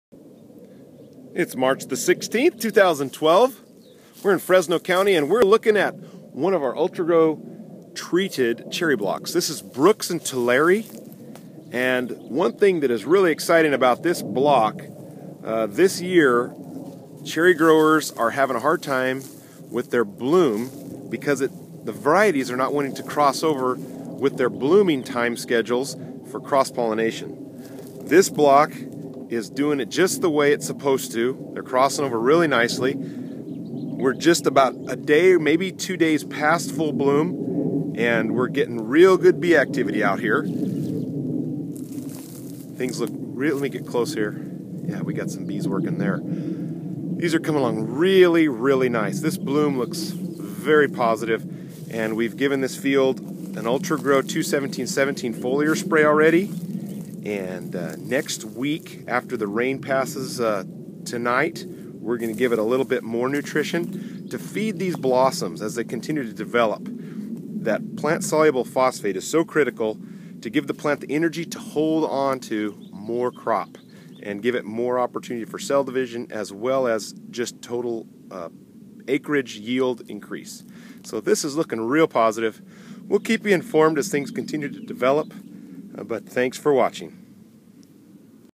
Marc Suderman #139 - Fresno county Brooks cherry: 3/16/12
This treatment block is at Full Bloom stage. The varieties are crossing over very well for cross-pollination.

The trees have been foliar treated with a "plant-ready" P & K nutrient and plant protectant to protect against low temperatures.